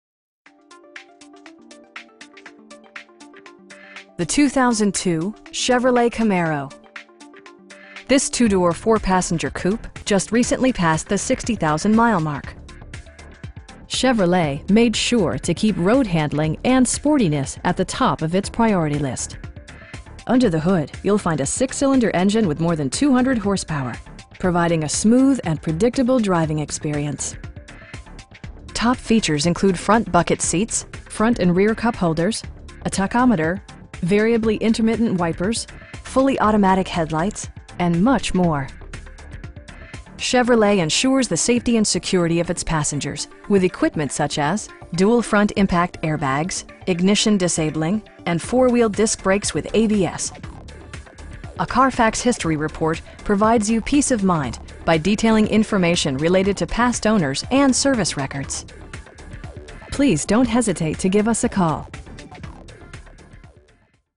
2002 Chevrolet Camaro CHECK OUT THESE LOOOOOW MILES....
7232 Ritchie Highway
in Glen Burnie, MD 21061

The 2002 Chevrolet Camaro. This 2 door, 4 passenger coupe just recently passed the 60,000 mile mark! Chevrolet made sure to keep road-handling and sportiness at the top of it's priority list. Under the hood you'll find a 6 cylinder engine with more than 200 horsepower, providing a smooth and predictable driving experience. Top features include front bucket seats, front and rear cupholders, a tachometer, variably intermittent wipers, air conditioning, fully automatic headlights, tilt steering wheel, and much more. Chevrolet ensures the safety and security of its passengers with equipment such as: dual front impact airbags, ignition disabling, and 4 wheel disc brakes with ABS. A Carfax history report provides you peace of mind by detailing information related to past owners and service records. We pride ourselves on providing excellent customer service. Please don't hesitate to give us a call.